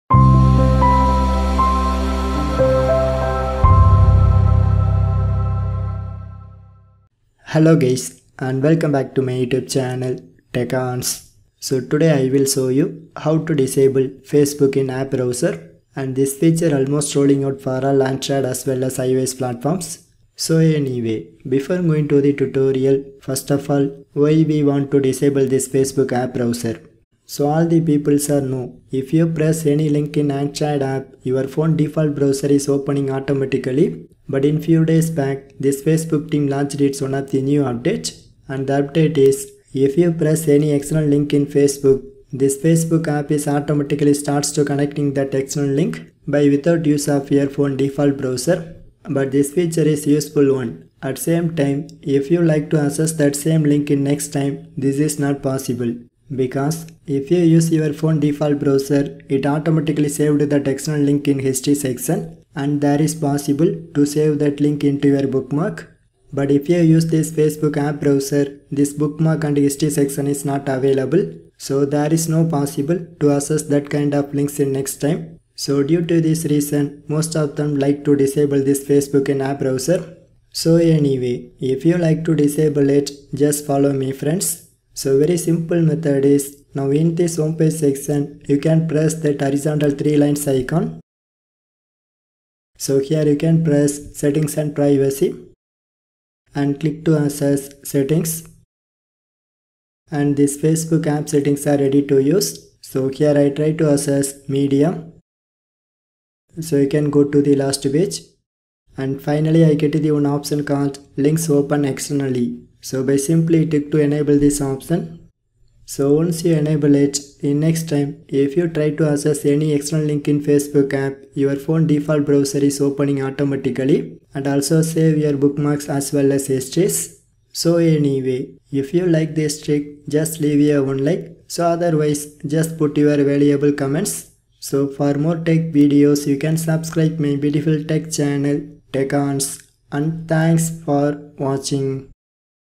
How to disable facebook in app browser | Embedded browser is disabled facebook problem
This tutorial is about how to disable facebook in app browser and fix fb embedded browser is disabled.

Always use 2023 released new update of app on your android mobile.

But this "Links open externally" option is not available and not working in ios based iphone and ipad devices.

During login process,if you get any problem just comment to me,i give some ideas to fix it.

This video has english subtitle at same time you can translate to tamil,kaise kare hindi,bangla telugu,kannada and malayalam languages.

How to solve logging into facebook from an embedded browser is disabled :

1.One of my subscriber ask why we want to turn off this feature?.

2.My point of view if you try to access any external link in android app,

3.your phone default browser is automatically opening.

4.But it saves your browsing history,so in next time if you try to access any website just use that history.

5.At same time if you enable this facebook app browser,it doesn't save your history and not able to add bookmarks.

6.So my best suggestion is,always disable this app browser in facebook app.

7.This same solution also works for fb lite app.